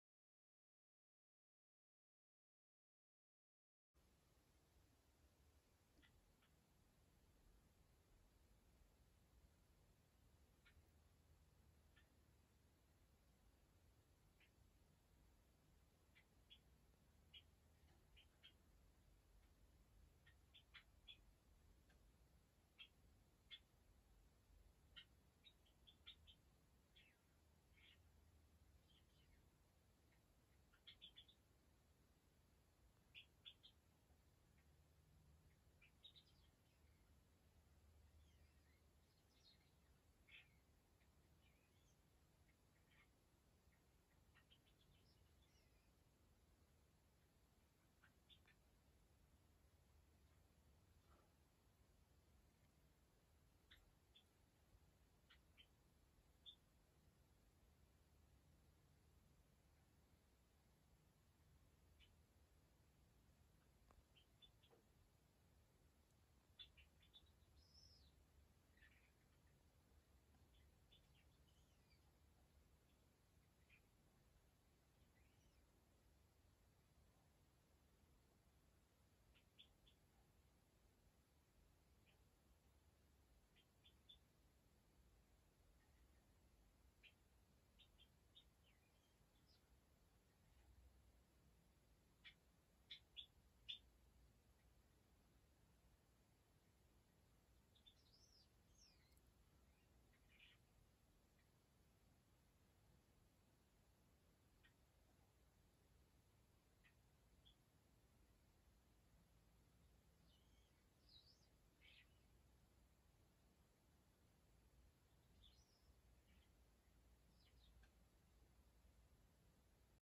（文鳥の日常vol.13）文鳥、もしかしてぐぜり？それともただのさえずり？
2021年10月22日。私の姿を見ると鳴き止んでしまうので、隣室から録音しました😊ぐぜりなのでしょうか？それともただのさえずりなのでしょうか？